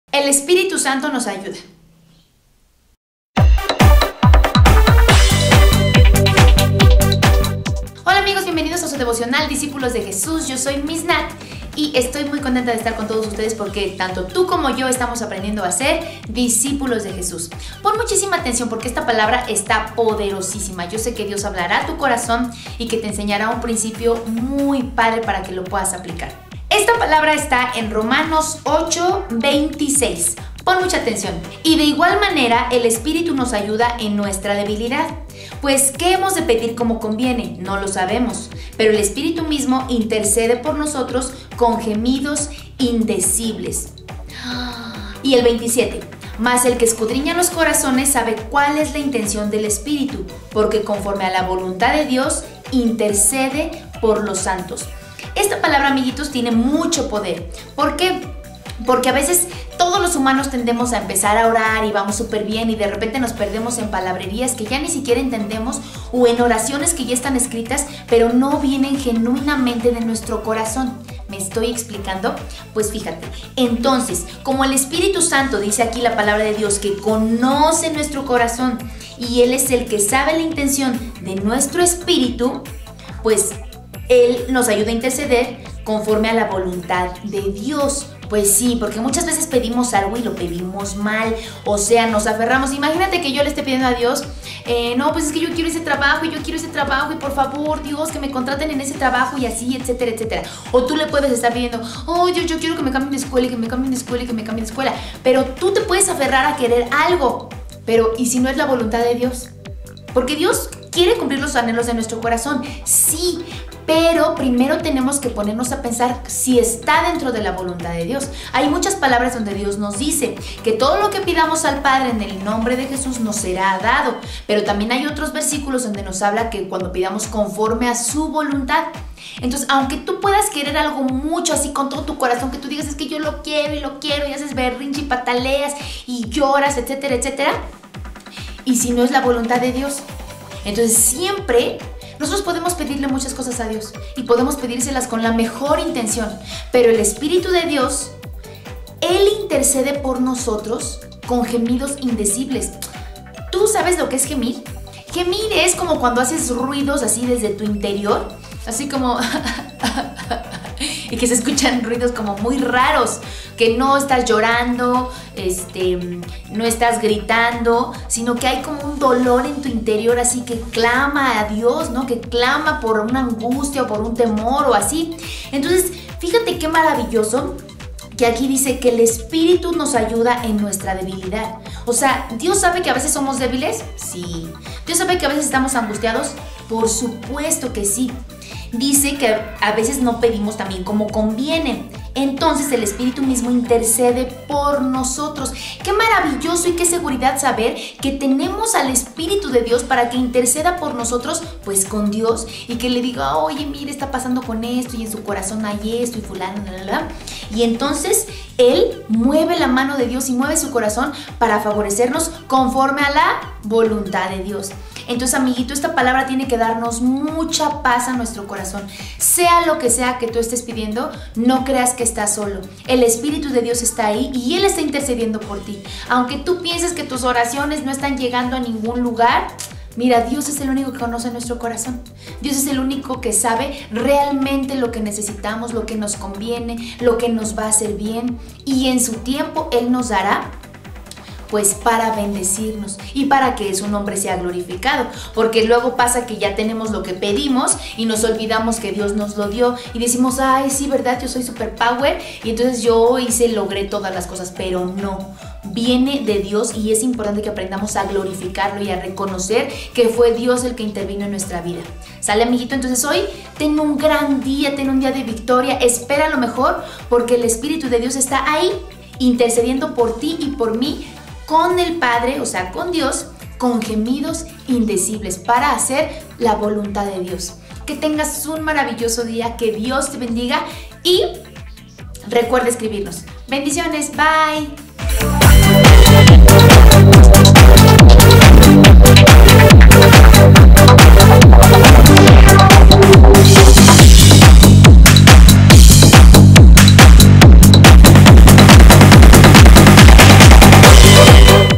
El Espíritu Santo es tu AYUDA 🙌. Devocional para niños Amy & Andy ❤️ Miss Nat
El Espíritu Santo nos ayuda. Devocional familiar Discípulos de Jesús Amy & Andy ❤️  Miss Nat
Romanos 8:26 qué hermoso es saber que el Espíritu Santo intercede por nosotros pues sabe lo que nos conviene.
Coméntanos lo que Dios habló a tu vida !!!